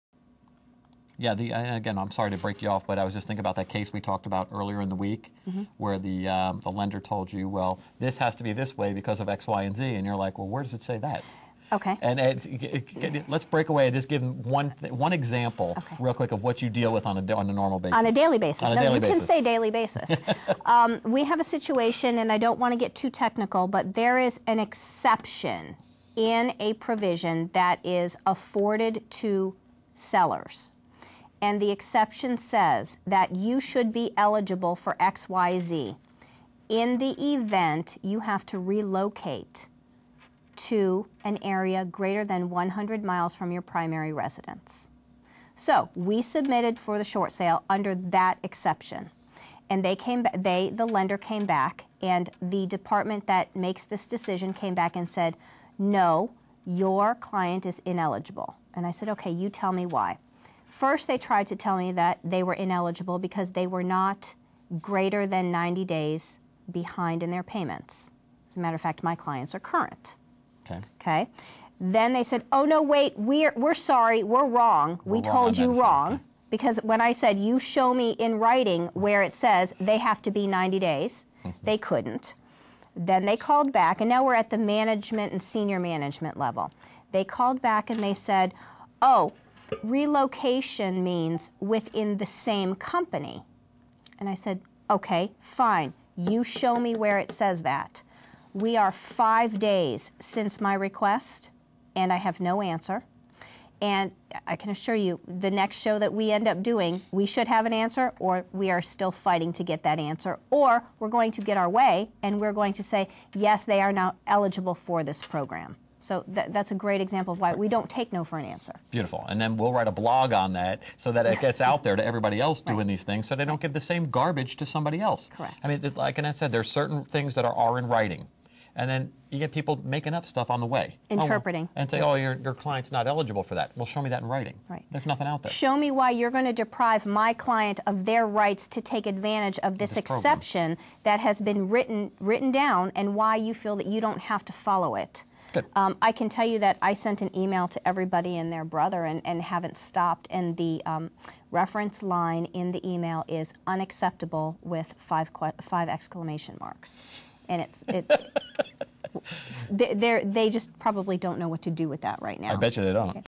Why does the lender state my client is ineligible?
We don't take no for an answer. Make sure it is in writing.